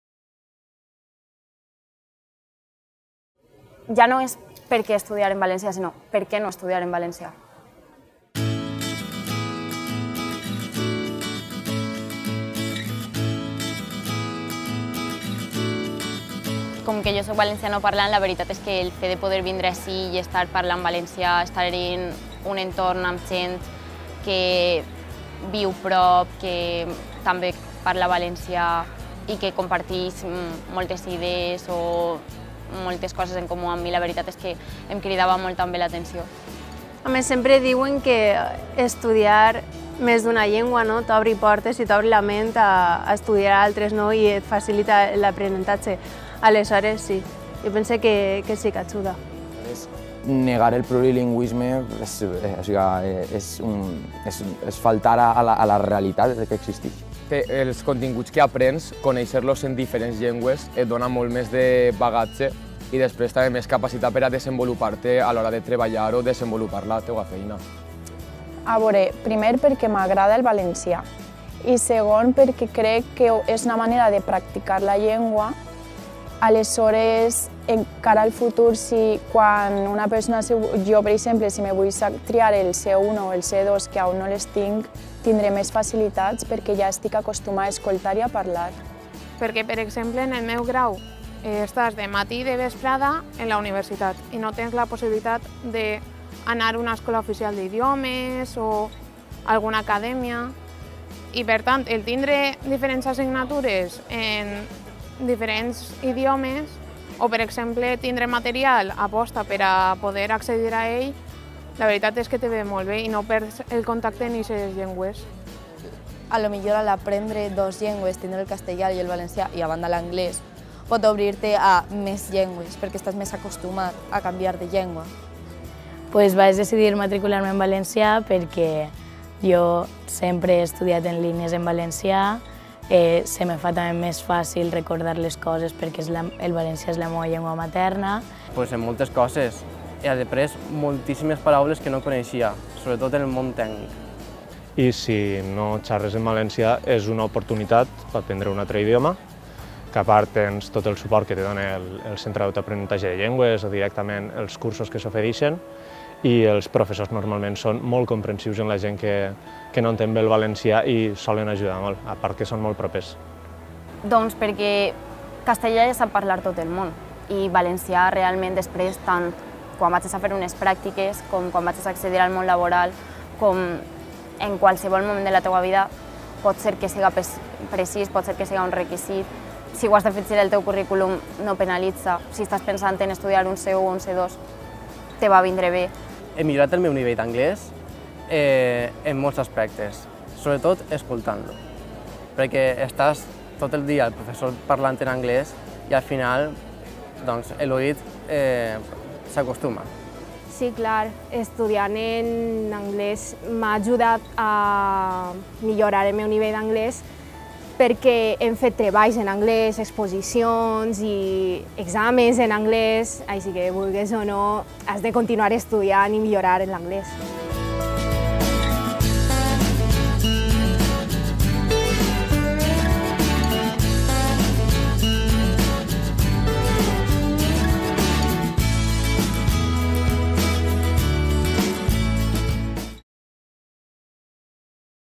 Valenciana i plurilingüe, la teua universitat (estudiantat)
L'estudiantat de la Universitat de València ens explica per què estudien en valencià i anglès. Amb nosaltres, estudia plurilingüe!